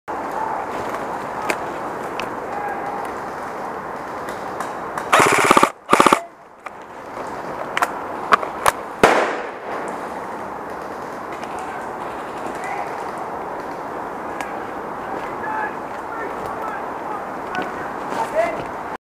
Why mid-cap mags are best in airsoft...
A very quick simple demonstration of why I've never used hi-cap magazines in my primary weapon.